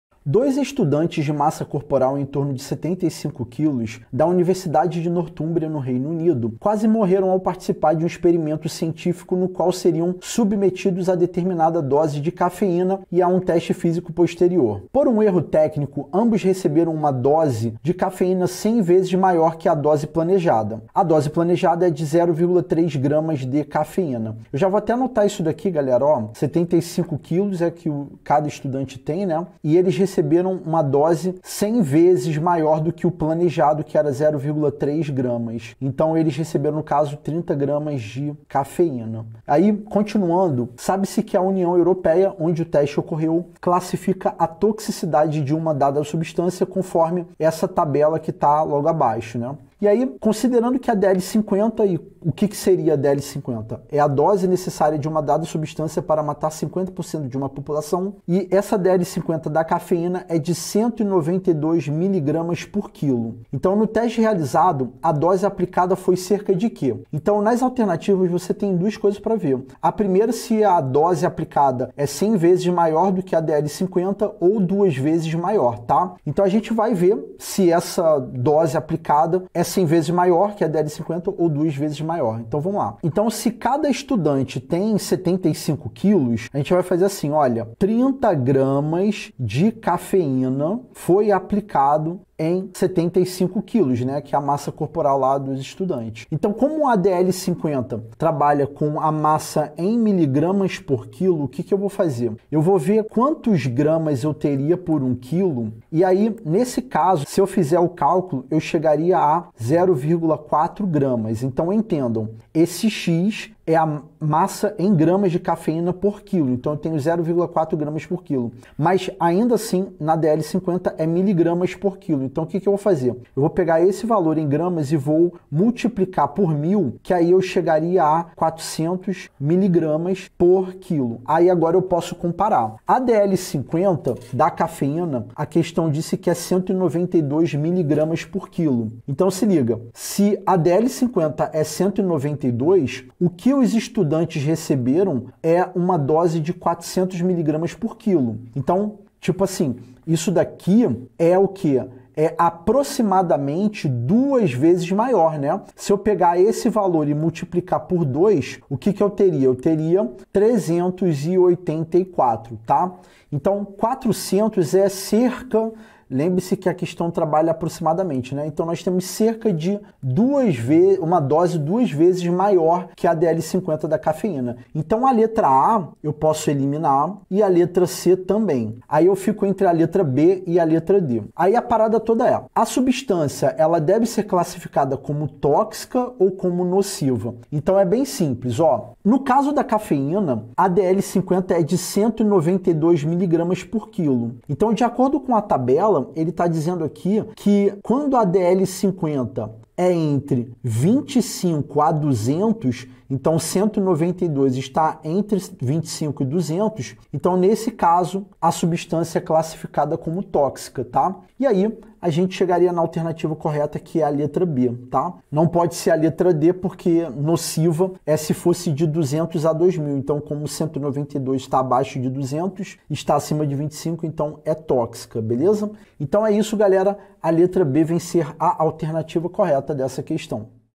[UNICAMP 2019/Q18] "Dois estudantes, de massa corporal em torno de 75 kg, da Universidade de Northum
Dois estudantes, de massa corporal em torno de 75 kg, da Universidade de Northumbria, no Reino Unido, quase morreram ao participar de um experimento científico no qual seriam submetidos a determinada dose de cafeína e a um teste físico posterior. Por um erro técnico, ambos receberam uma dose de cafeína 100 vezes maior que a dose planejada. A dose planejada era de 0,3 g de cafeína, equivalente a três xícaras de café. Sabe-se que a União Europeia, onde o teste ocorreu, classifica a toxicidade de uma dada substância conforme tabela a seguir. Considerando que a DL50 – dose necessária de uma dada substância para matar 50% de uma população – da cafeína é de 192 mg/kg, no teste realizado a dose aplicada foi cerca de a) 100 vezes maior que a DL50 da cafeína, substância que deve ser classificada como nociva. b) duas vezes maior que a DL50 da cafeína, substância que deve ser classificada como tóxica. c) 100 vezes maior que a DL50 da cafeína, substância que deve ser classificada como tóxica. d) duas vezes maior que a DL50 da cafeína, substância que deve ser classificada como nociva.